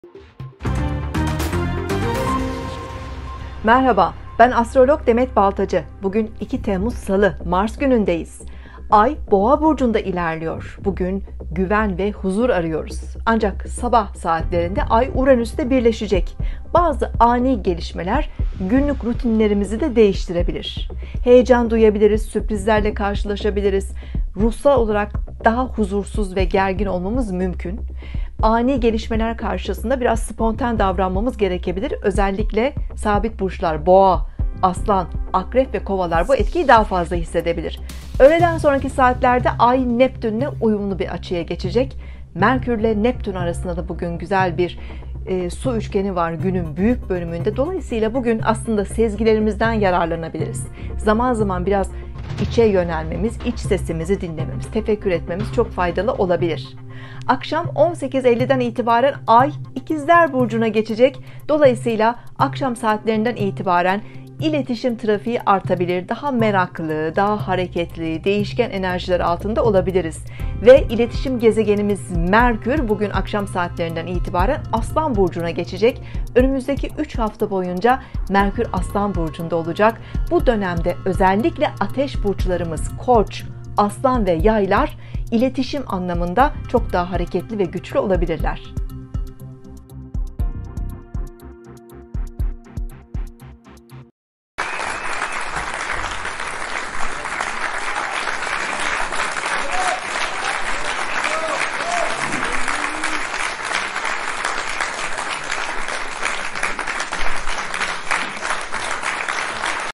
Bugün 2 Temmuz 2024 Salı, Günlük Burç Yorumu. Astrolog Demet Baltacı ile astroloji burçlar haber
Bugün 2 Temmuz 2024 Salı, Günlük Burç Yorumu. Astrolog Demet Baltacı astroloji burçlar haberler.